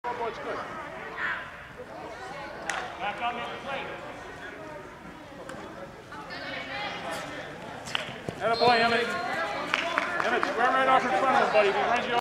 56 Silky Hogan-Hurricanes throw to the side. Emmit makes the stop.
Ravens Gold Highlights Winter 2
U13-Flag Football Game 1
Saturday 12/26/20 9:00 am
Sportsplex, Bedford NH
Ravens Gold 6 vs 28 Hurricanes